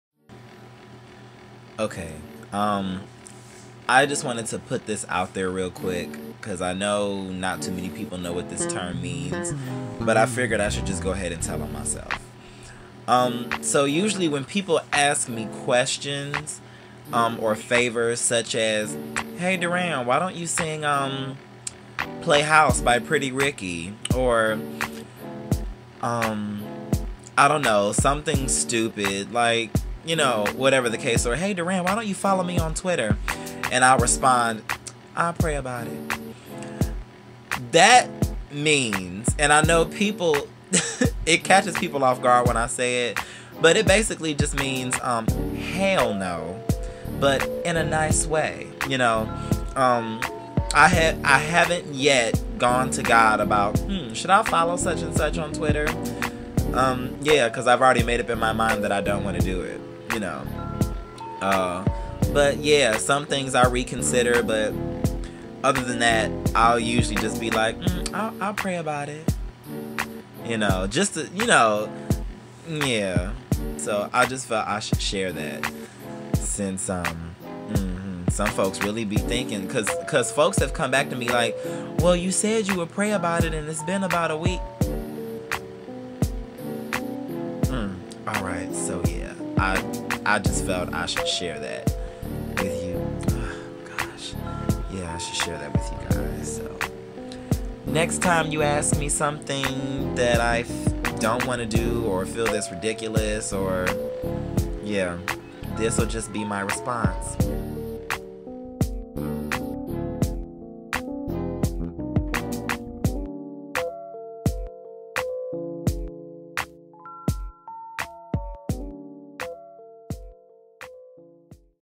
I'll Pray About It
Just making sure you guys know what I mean when I say this.